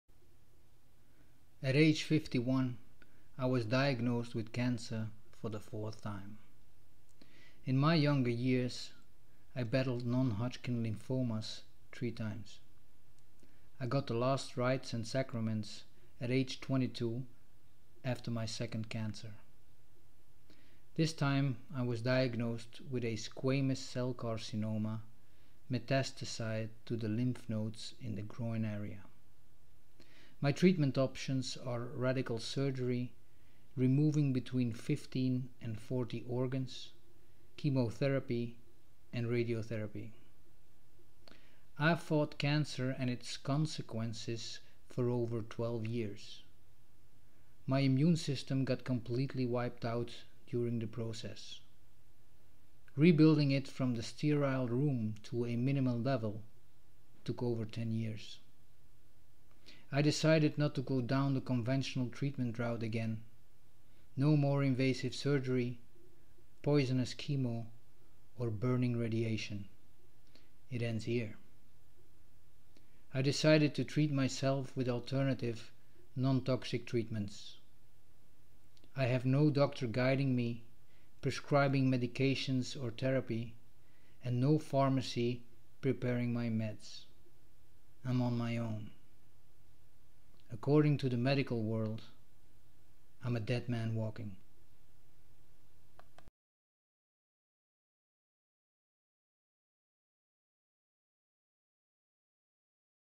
My 4th cancer - no more conventional treatment.
At age 51, I was diagnosed with cancer for the 4th time.
In my younger years, I battled non-Hodgkin lymphomas 3 times. I got the last rites and sacraments at age 22 after my second cancer.
This time, I was diagnosed with a Squamous Cell carcinoma metastasized to the lymph nodes in the groin area.
My treatment options are radical surgery, removing between 15 and 40 organs, chemotherapy, and radiotherapy.
I fought cancer and its consequences for over 12 years. My immune system got completely wiped out during the process. Rebuilding it from the sterile room to a minimum level took over 10 years.
I decided not to go down the conventional treatment route again. No more invasive surgery, poisonous chemo, or burning radiation. It ends here.
I decided to treat myself with alternative, non-toxic treatments. I have no doctor guiding me, prescribing medication or therapy, and no pharmacy preparing my meds. I'm on my own.
According to the medical world, I'm a dead man walking.